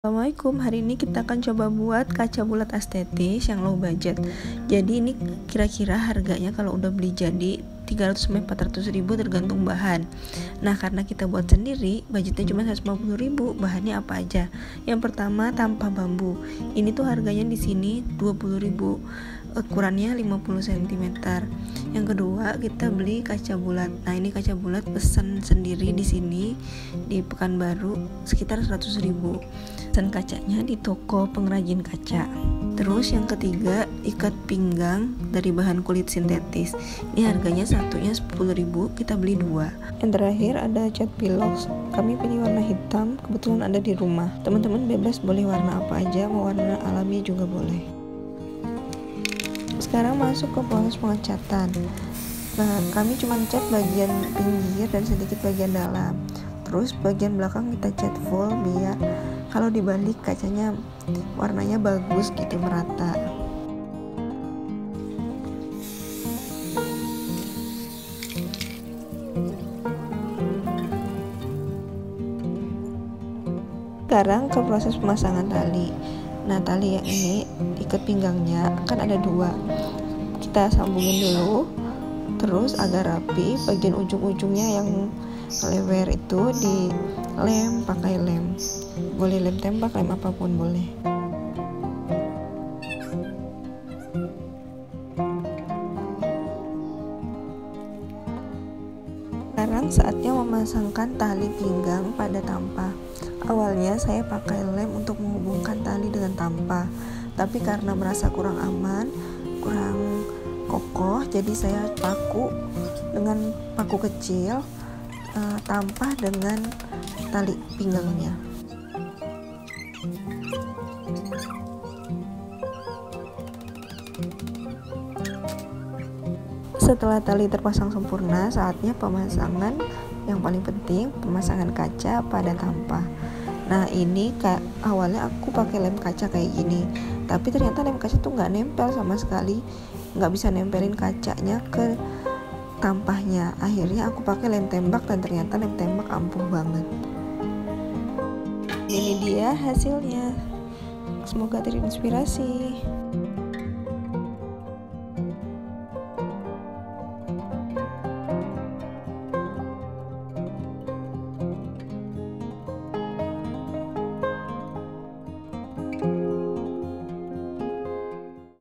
DIY Room Decor : Cermin Bulat Aesthetic Low Budget
Kaca bulat aestetis gini biasanya harganya 300-400rb tergantung bahannya. Tapi kalau bikin sendiri kita bisa hemat hingga kurang dari 1/2 harga beli jadi.

bahannya pun mudah
- kaca bulat pesan di tukang kaca
- tampah bulat
- ikat pinggang sd
- lem tembak

yuk bikin sendiri kaca bulat aestheticmu..